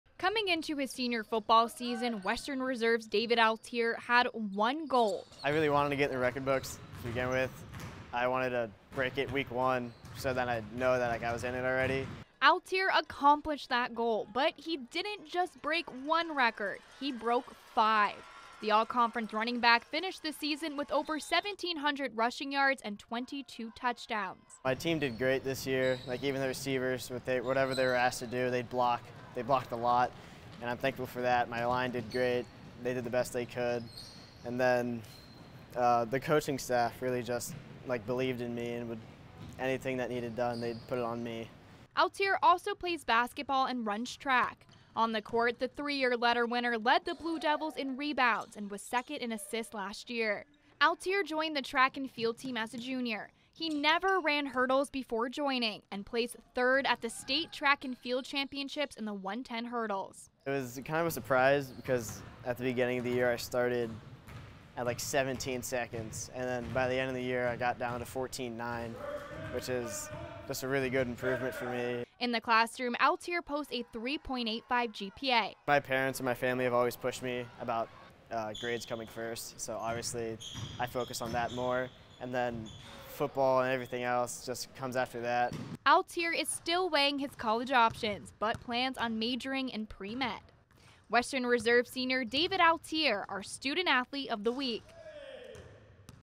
Western Reserve standout running back, record holder named 'Student Athlete of the Week'
David Altiere finished his senior season with over 1700 rushing yards and 22 touchdowns